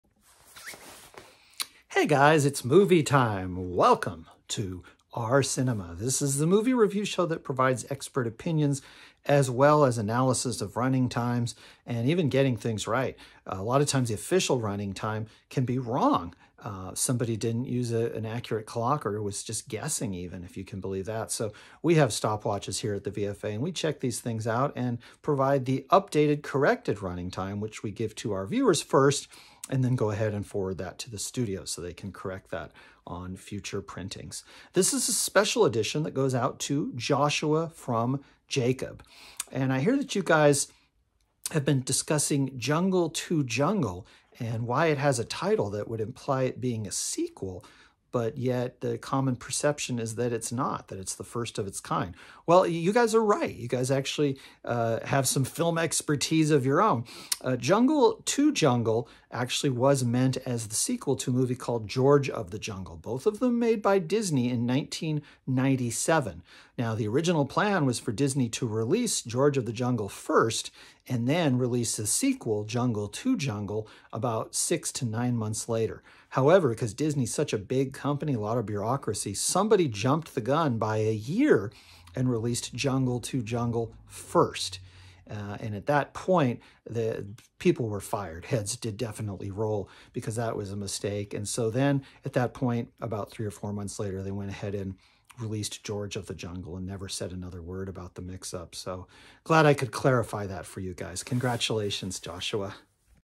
Film Buff Gregg Turkington Finally Settles the Jungle 1 Jungle Controversy!
Owner of the VFA, and Master Film Buff settles the controversy of the lost movie Jungle 1 Jungle, which turns out to actually be not so lost after all! Thank you Mr. Turkington for this wonderful personalized video!